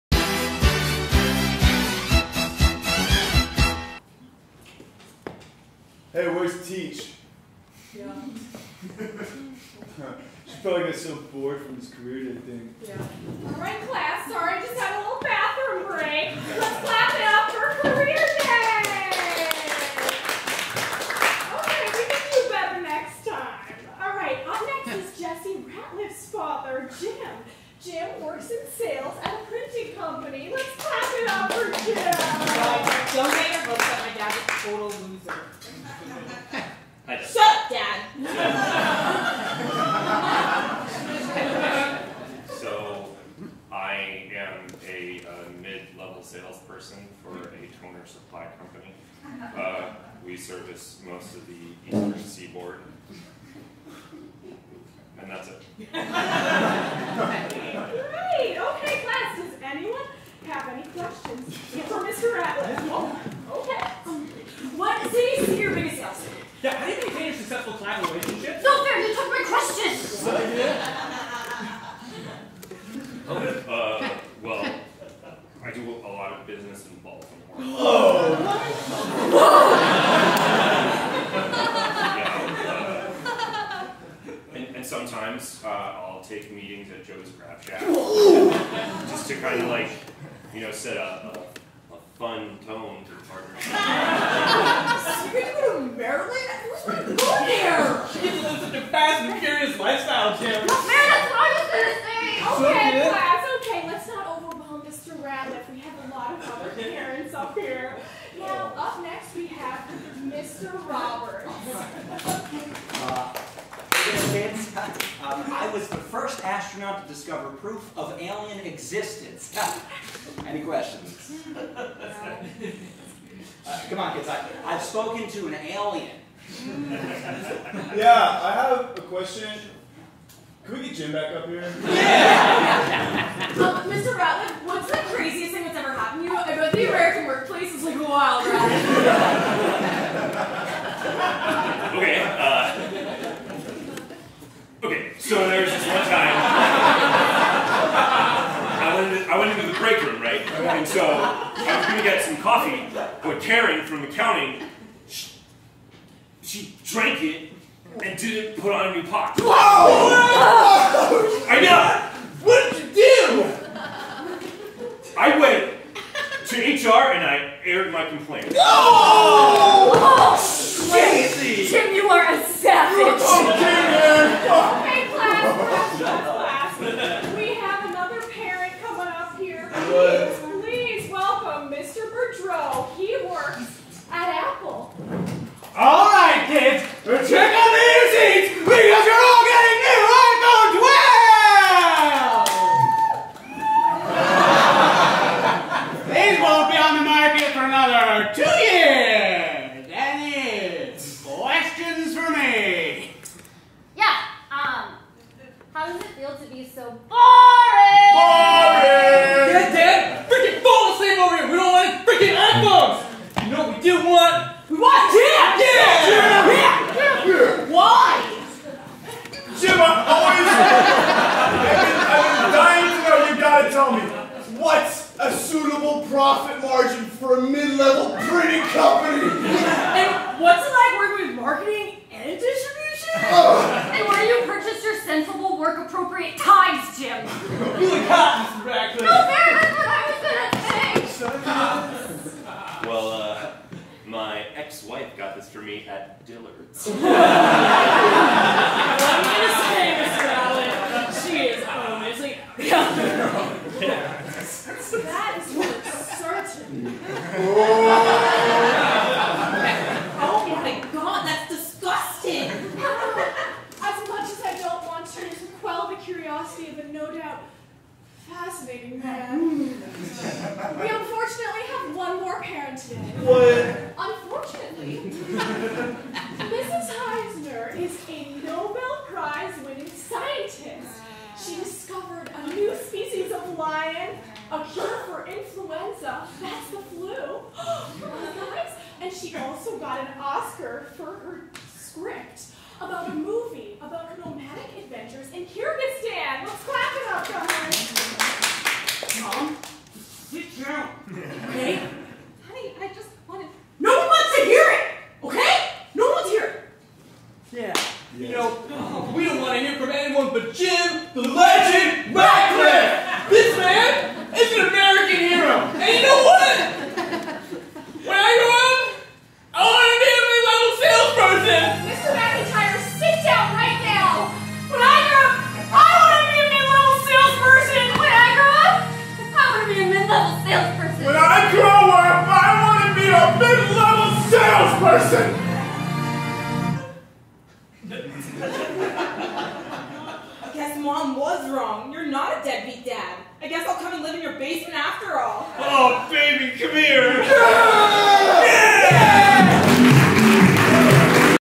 Career Day
Discotheque Support
3/3/20 Super Sketch Tuesday at The PIT, NYC

Written by HaleyJane Rose
Directed by Taryn O'Connor

Performers:
Rachael Burke
Angela Batuure
J. Steven Madura
Brittany Miggs
Dean Preston
Jennifer Haining
Dante Gianetta
Tom Kalnas
Allyn Engle
HaleyJane Rose